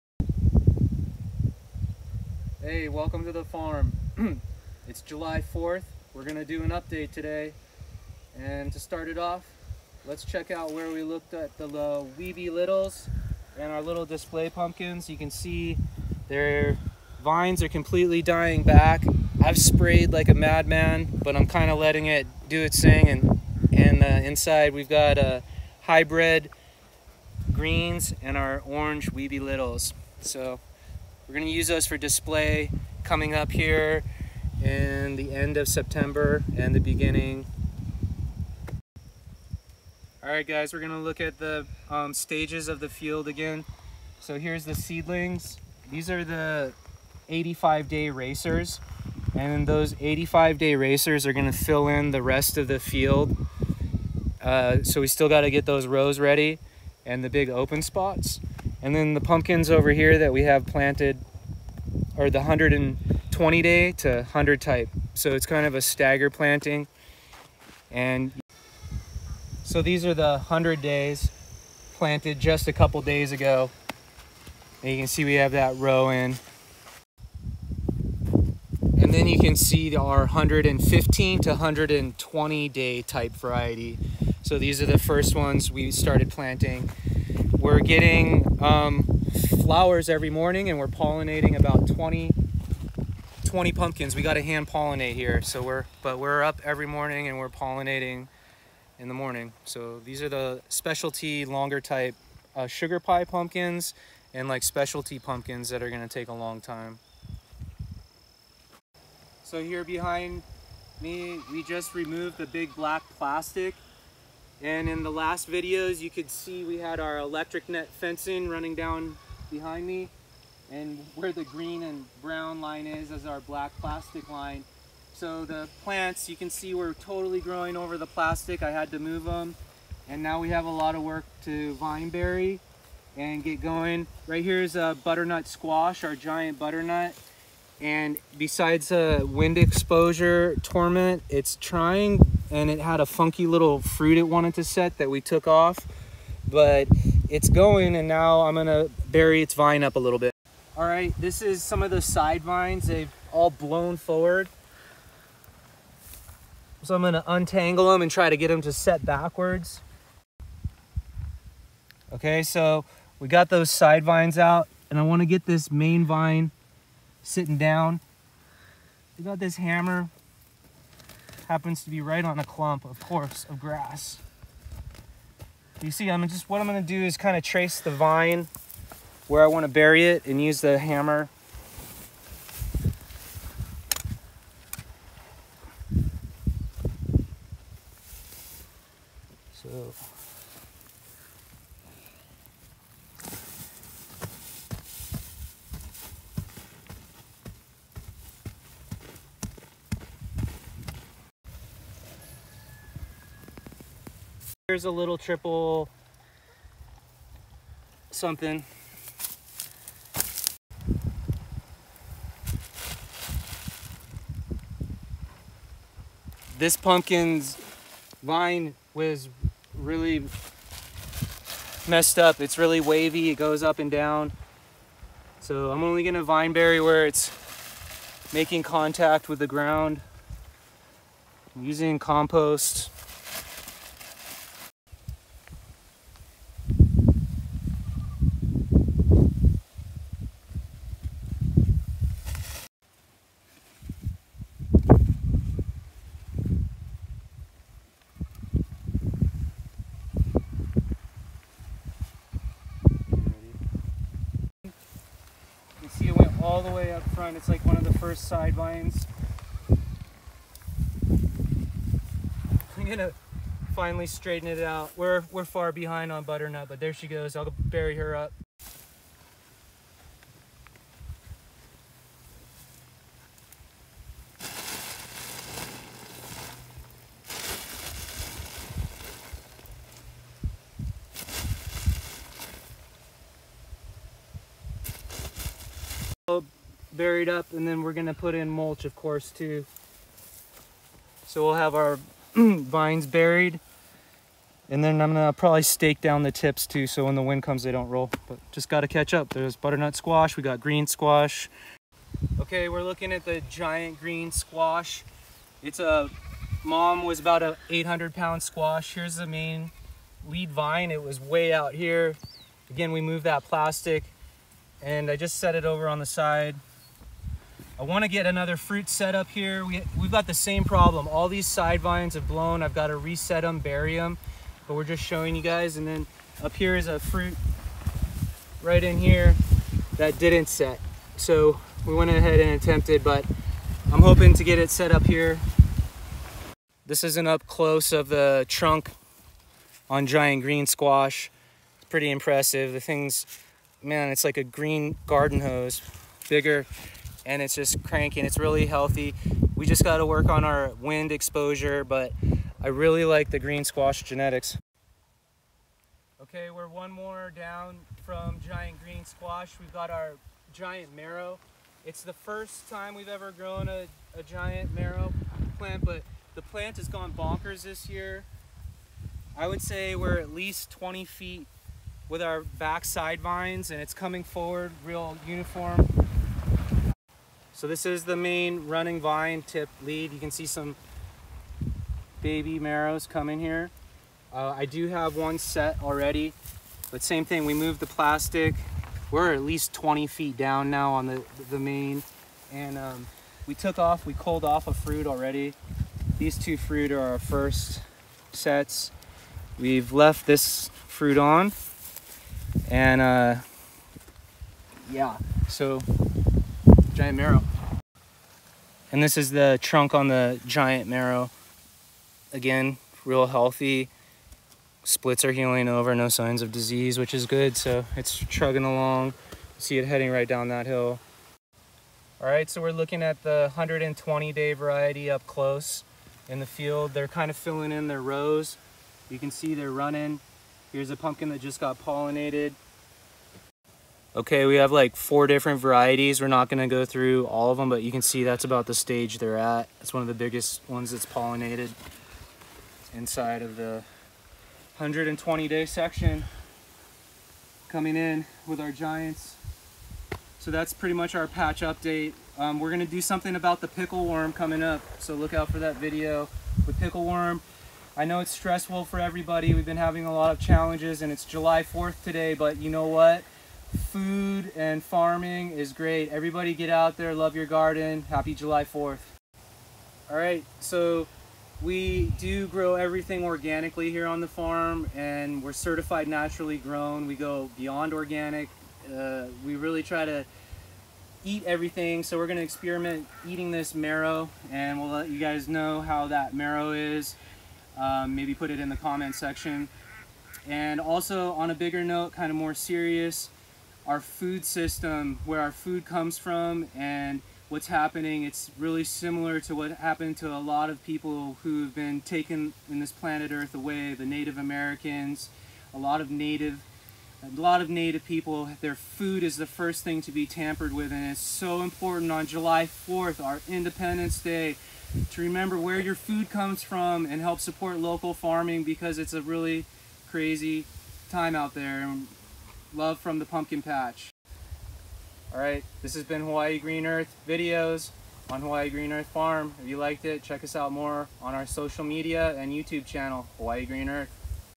Pumpkin patch Hawaii update July 4 2022 big island Hawaii pumpkins 🎃
We’ll it’s July 4th already we are still playing catch-up in the field … we take a look at what’s growing … Giant veggies and field pumpkins 🎃 getting ready to plant the 85 day racers ….